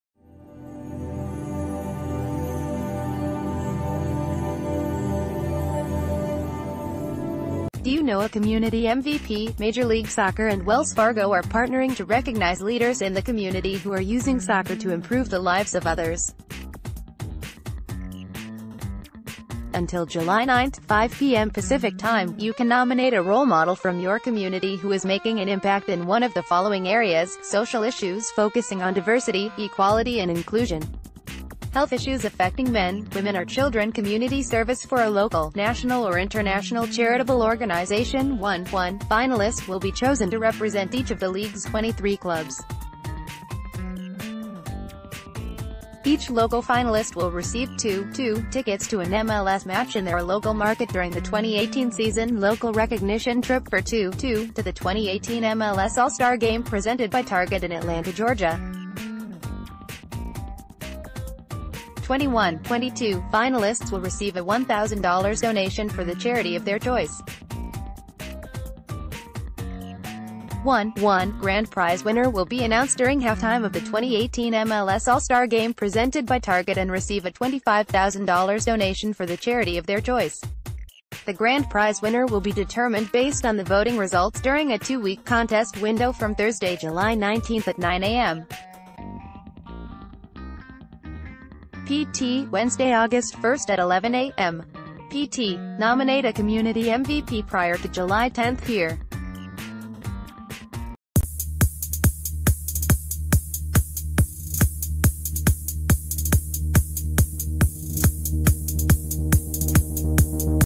Breaking News | Nominate a Community MVP today!
Do you know a Community MVP?  Major League Soccer and Wells Fargo are partnering to recognize leaders in the community who are using soccer to improve the lives of others. Until July 9 (5:00PM PT), you can nominate a role model from your community who is making an impact in one of the following areas: Social issues focusing on diversity, equality and inclusion Health issues affecting men, women or children Community service for a local, national or international charitable organization One (1) f...